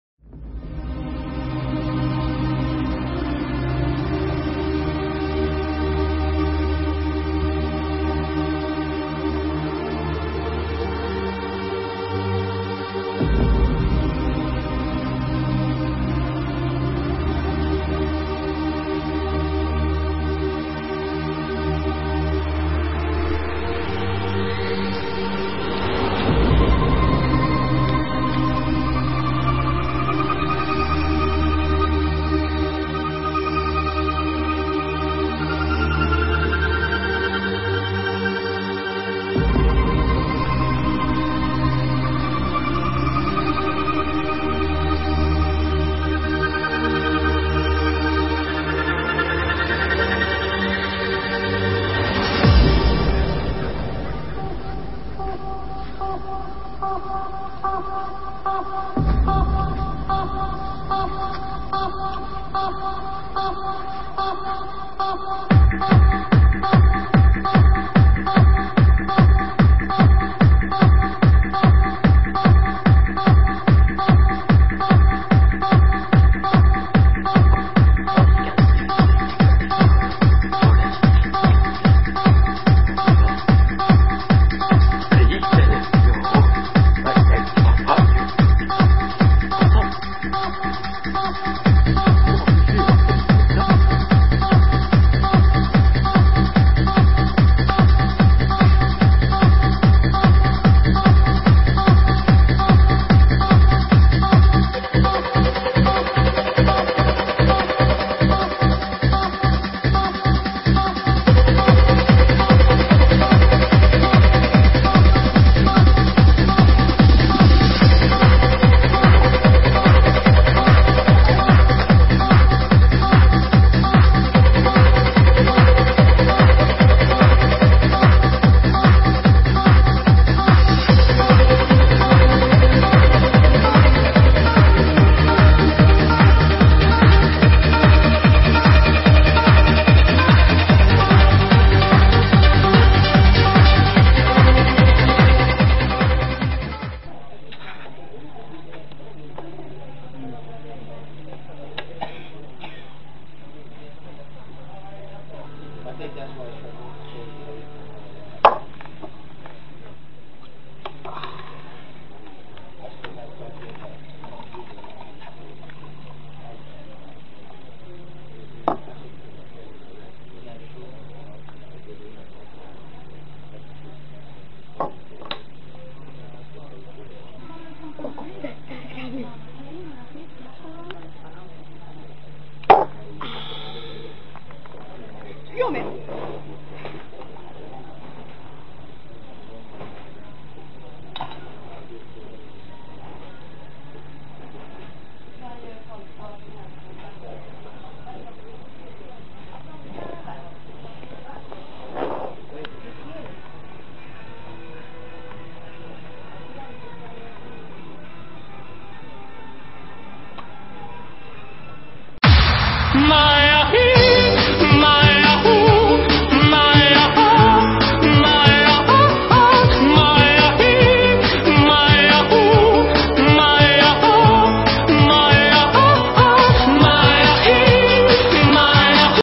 André på Ica i Vänersborg
André gillar Ica Kvantum i Vänersborg. Det är ännu bättre med hög technomusik i hörlurarna.